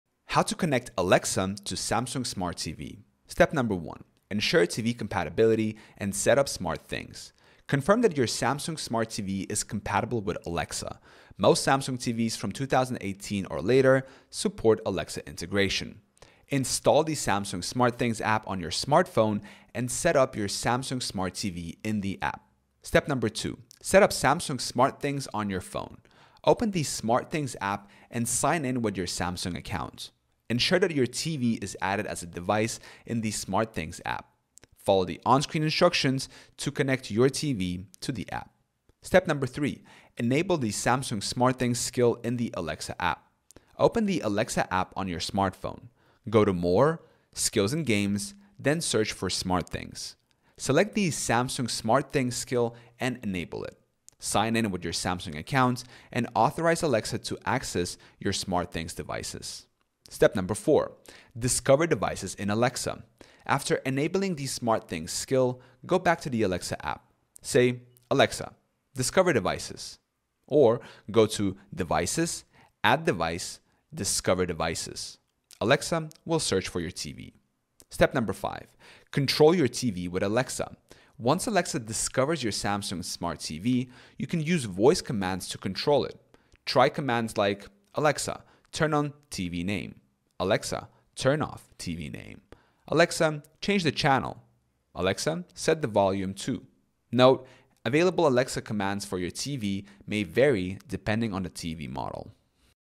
How to Connect Alexa to Samsung Smart TV
In this video, you'll learn how to connect your Alexa to your Samsung Smart TV for hands-free control using voice commands. Whether you want to turn on your TV, change the channel, or adjust the volume, Alexa makes it easy to control your Samsung TV with just your voice.

00:00 Introduction
00:03 Step 1: Ensure TV Compatibility and Set Up SmartThings
00:23 Step 2: Set Up Samsung SmartThings on Your Phone
00:39 Step 3: Enable the Samsung SmartThings Skill in the Alexa App
01:00 Step 4: Discover Devices in Alexa
01:17 Step 5: Control Your TV with Alexa
01:37 Note